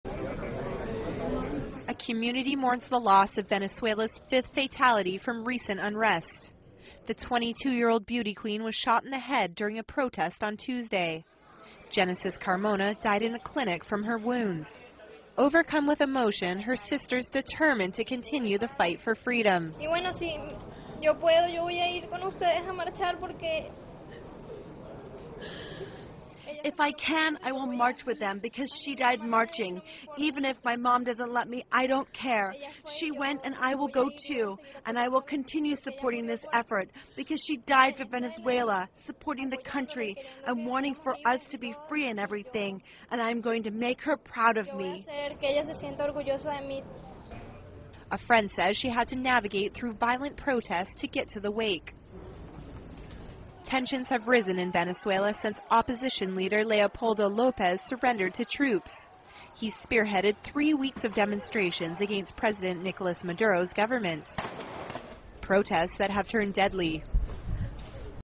Venezuela Beauty Queen Shot and Killed
A community mourns the loss of Venezuela's 5th fatality from recent unrest.

The 22 year-old beauty queen was shot in the head during a protest on Tuesday. Genesis Carmona died in a clinic from her wounds.

Overcome with emotion, her sister's determined to continue the fight for freedom:

(SOUNDBITE) (Spanish) ALEJANDRA CARMONA, VICTIM'S SISTER, SAYING:

"If I can, I will march with them, because if she died marching... even if my mom doesn't let me, I don't care. She went and I will go too. And I will continue supporting this effort, because she died for Venezuela, supporting the country, and wanting for us to be free and everything. And I am going to make her proud of me."

A friend says she had to navigate through violent protests to get to the wake.

Tensions have risen in Venezuela since opposition leader Leopoldo Lopez surrendered to troops.

He's spearheaded three weeks of demonstrations against President Nicolas Maduro's government.

Protests that have turned deadly.